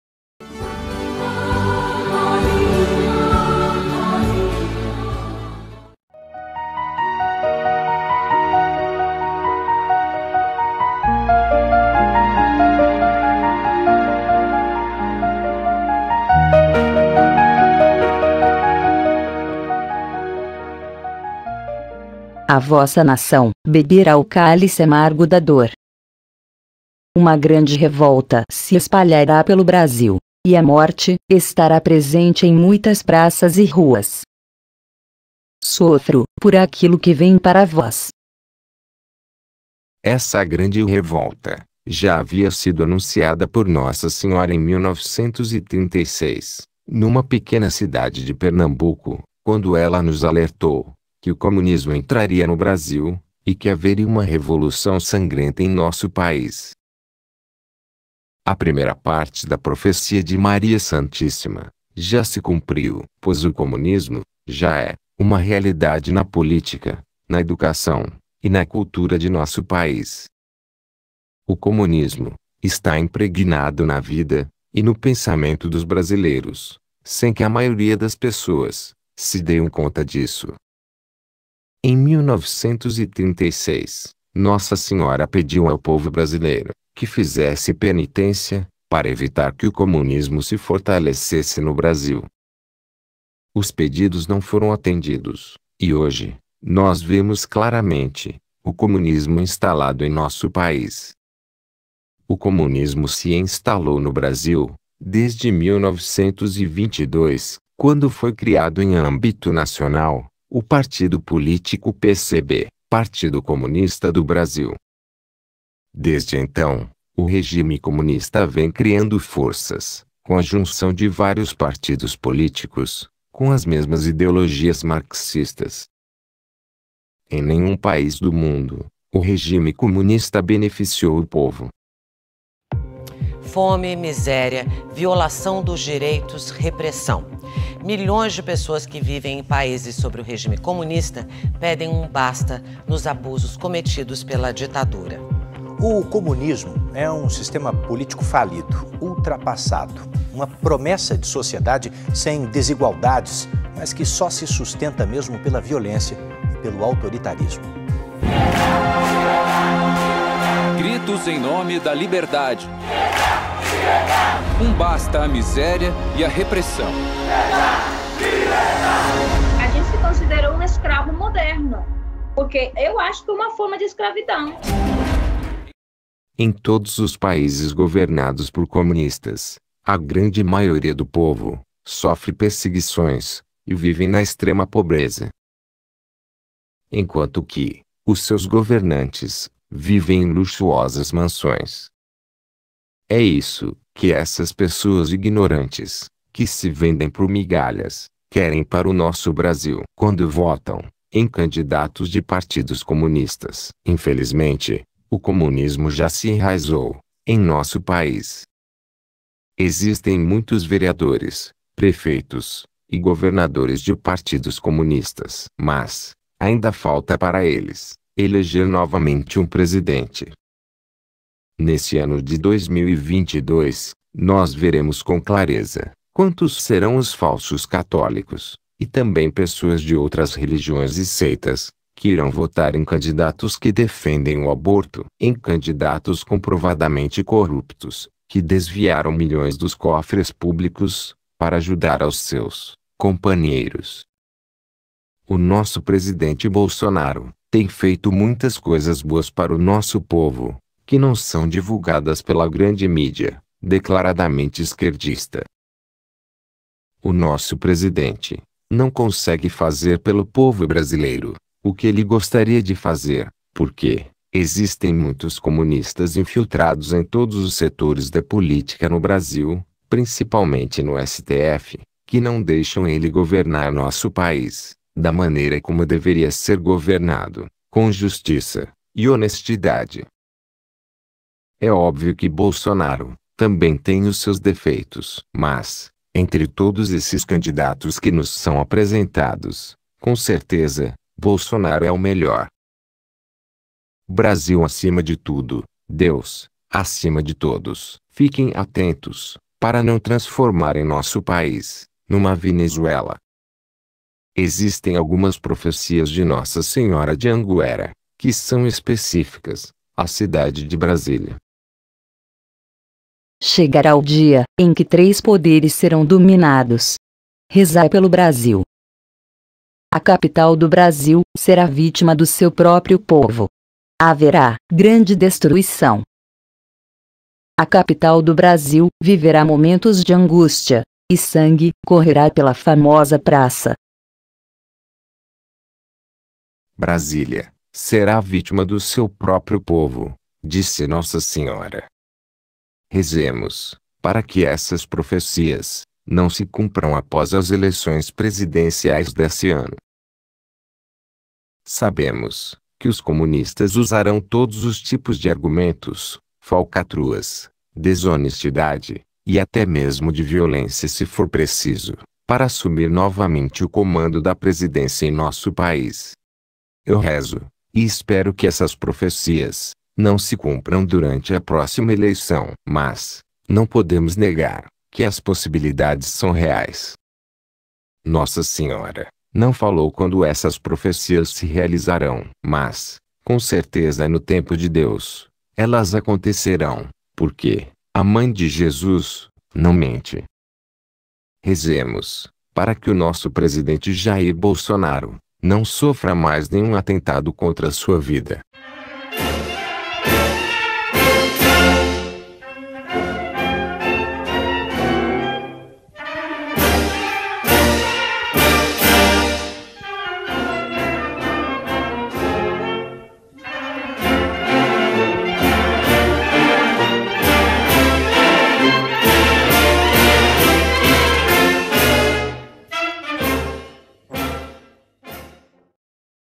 Uma grande REVOLTA se espalhará pelo Brasil
"A vossa nação beberá o cálice amargo da dor. Uma grande revolta se espalhará pelo Brasil e a morte estará presente em
muitas praças e ruas".